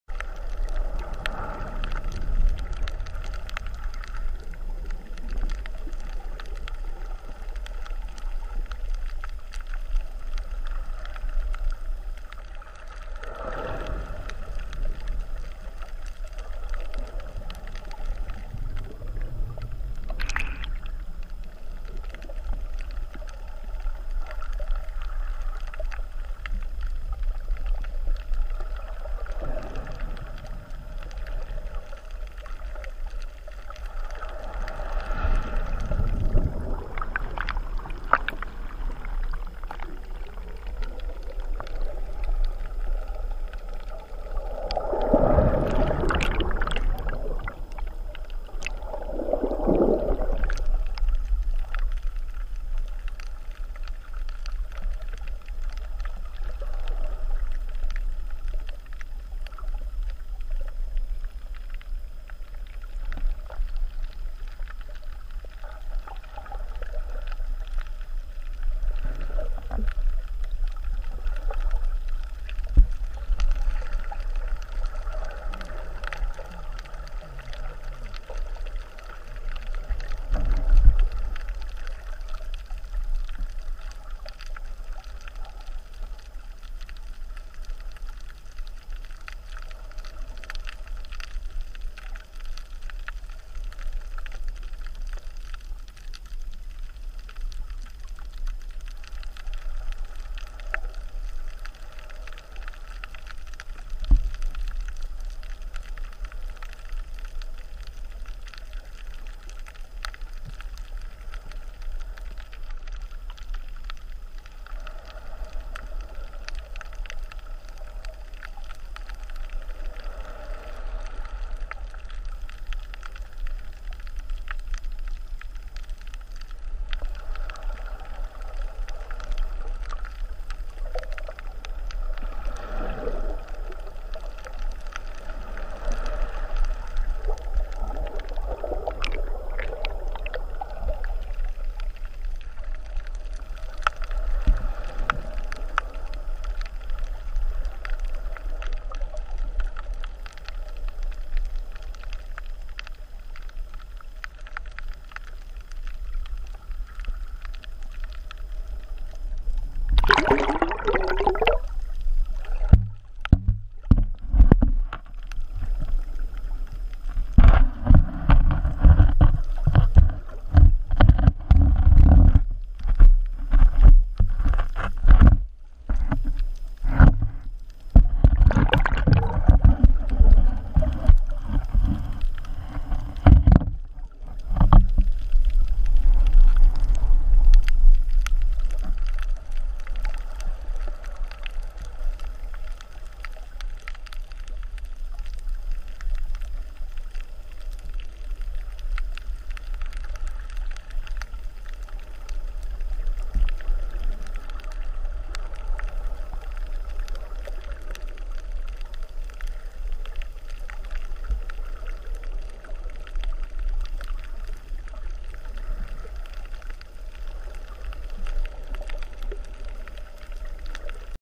Tiny Bubbles
Swimming very close to a reef on Oahu's North Shore during tidal surges.  Lots of reef aeration and young fish hiding out in the churn.  Shot with GoPro Hero3 Black at 1080p/60fps mounted to 4' telescoping pole. Edited with CineForm Studio (free app) to apply Protune effect.